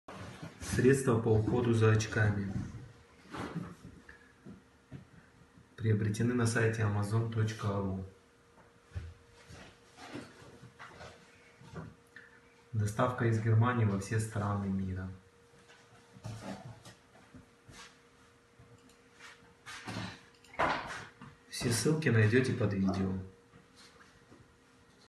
Средство для ухода за очками
Незаменимая вещь для тех, кто носит очки, пользуется фото-и видеокамерами, смартфонами и прочим, имеющим линзы или стеклянную поверхность. Быстро и эффективно устраняет с поверхности линз любые загрязнения, включая жирные следы от пальцев. Не оставляет разводов. Зимой препятствует запотеванию очков при входе с улицы в тёплое помещение. Средства по уходу за очками на Амазоне за рубли. Есть доставка по всей территории СНГ.